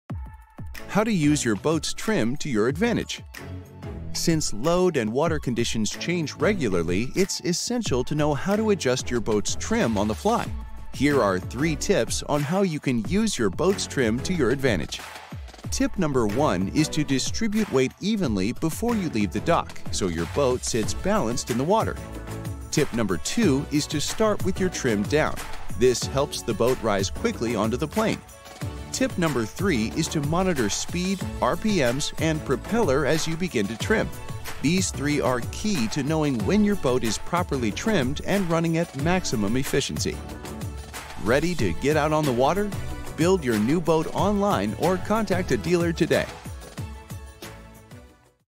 How to Use Your Boat’s Trim to Your Advantage
Boats handle best when they run parallel with their at-rest waterline — you’ll notice a more comfortable ride, increased speed and more efficient gas mileage. The bow of your boat meets the water at a specific angle — and that angle has a direct correlation to how efficiently you operate your vessel.